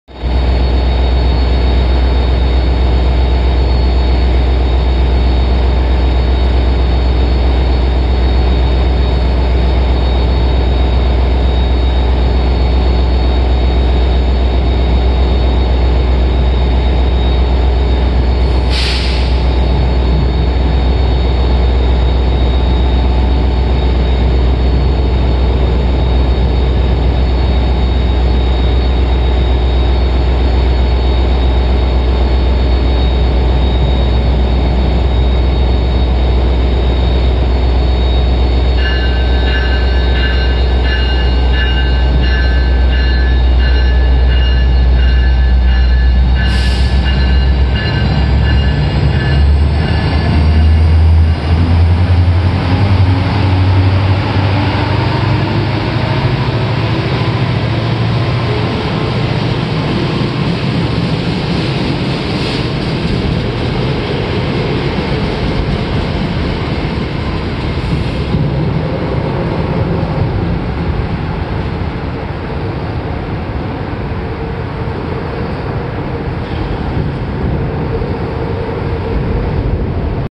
New Jersey Transit - Alstom PL42-AC #4032 departing Secaucus Junction
New Jersey Transit Alstom PL42-AC #4032, operating as Pascack Valley Line Train #1629 to Spring Valley, departing Secaucus Junction.

This was recorded with an LG V30.